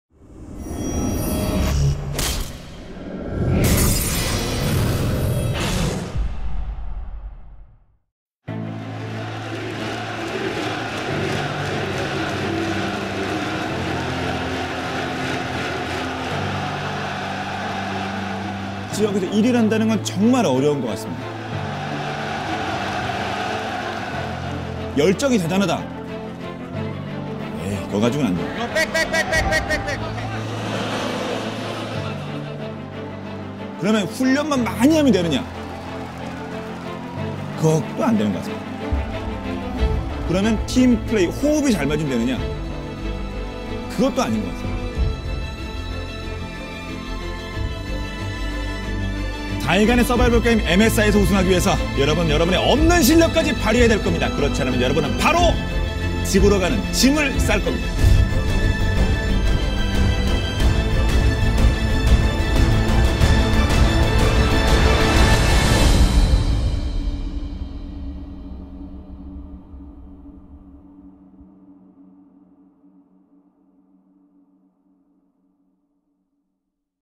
Mid-Season Invitational 2015: o que define um campeão
Quando toda equipe na competição é uma campeã, é preciso mais dos vencedores para clamar o  título. O Mid-Season Invitational começa no dia 7 de maio.

Para mais cobertura do Mid-Season Invitational, incluindo a programação, resultados, situação e análises, visite

Participe das discussões no Twitter, Tuíte
para @lolesportsbr